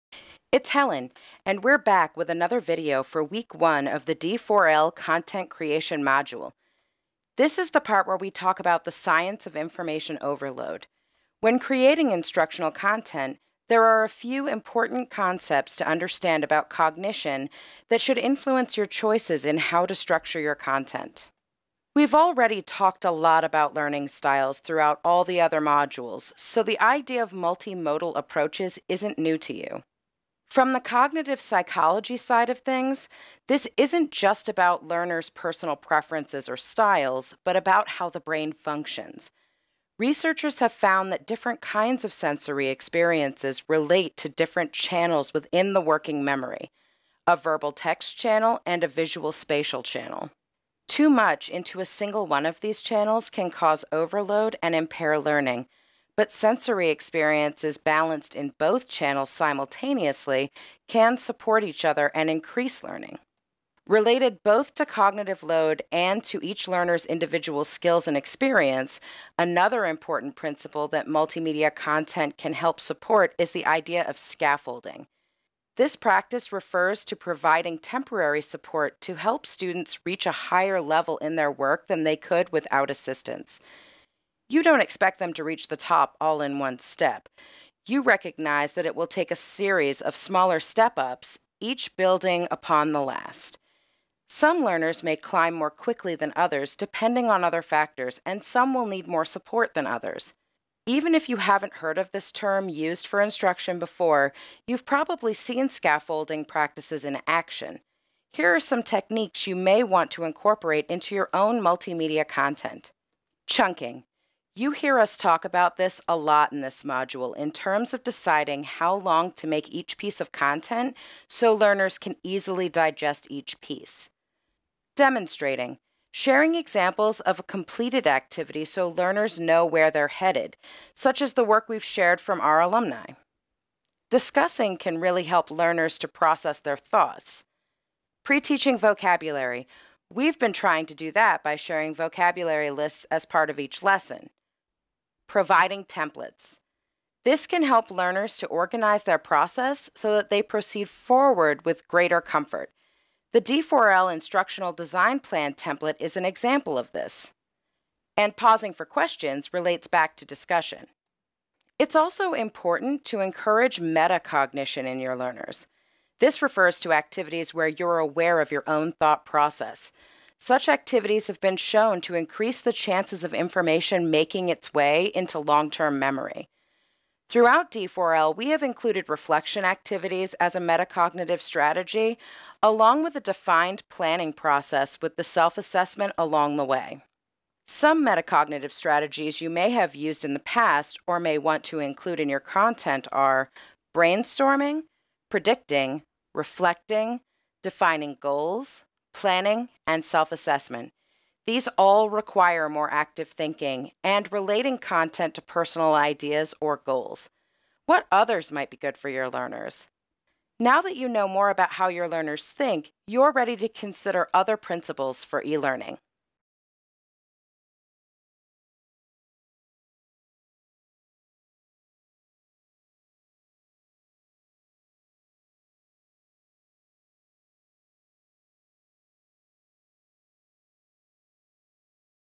D4L Content Week 1: Cognition
Helen Linda discusses instructional strategies that can help with learners' cognition for the Content Creation Module, part of the series for the Design for Learning program, teaching library workers how to teach online.